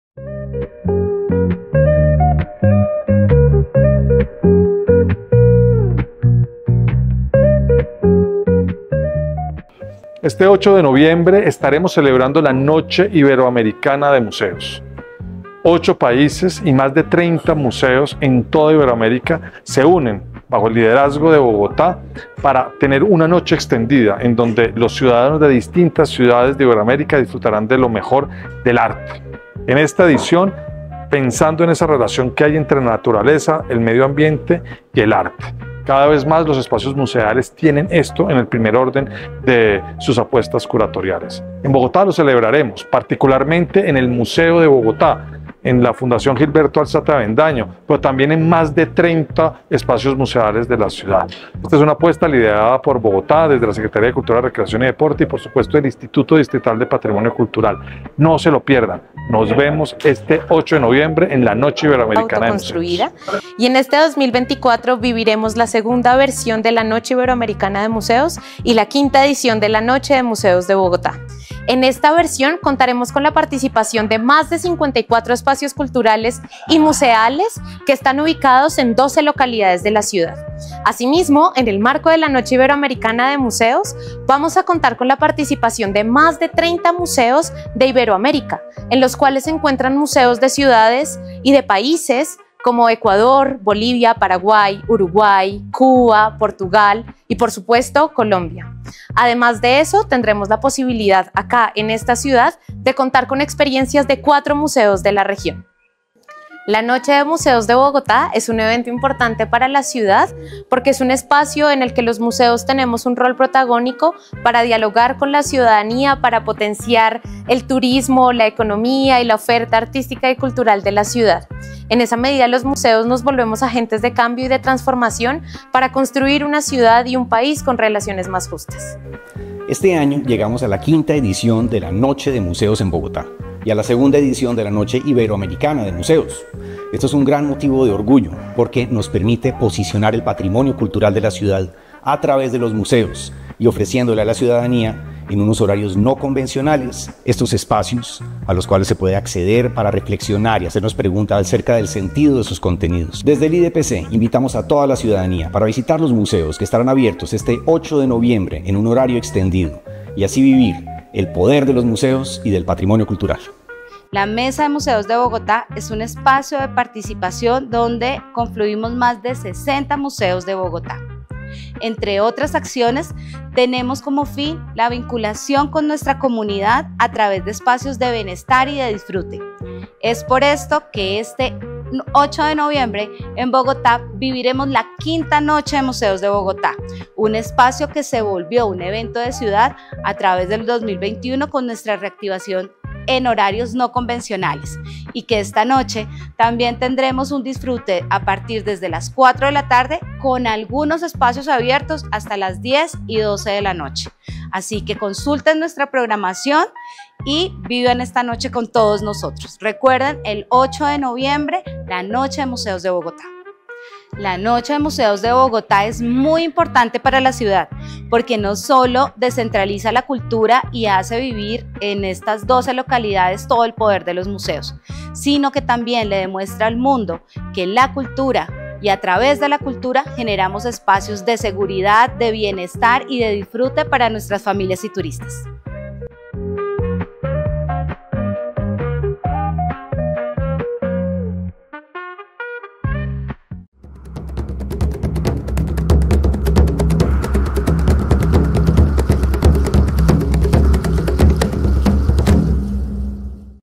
V Noche de Museos de Bogotá y la II Noche Iberoamericana de Museos
En esta edición especial de Quira Medios, celebrando nuestra columna número 100, te llevamos a un recorrido único por la Noche de Museos Bogotá 2024, un evento que resalta la importancia de los museos como espacios de aprendizaje, memoria y encuentro en la ciudad y en toda Latinoamérica. Conversamos con destacados líderes de la cultura en Bogotá, quienes comparten su visión sobre el papel de los museos en la transformación social y su poder para conectar a la ciudadanía con el arte y el patrimonio histórico.

Escucha las palabras de Santiago Trujillo, Secretario de Cultura, Eduardo Mazuera, Director del IDPC, Sandra Solano, Coordinadora de la Mesa de Museos, y Alejandra Romero, de la Gerencia del Museo de Bogotá, quienes nos explican la relevancia de esta iniciativa y lo que podemos esperar de esta noche tan especial.

Lee nuestra columna de :V Noche de Museos de Bogotá y la II Noche Iberoamericana de Museos 👇👇👇👇👇👇👇👇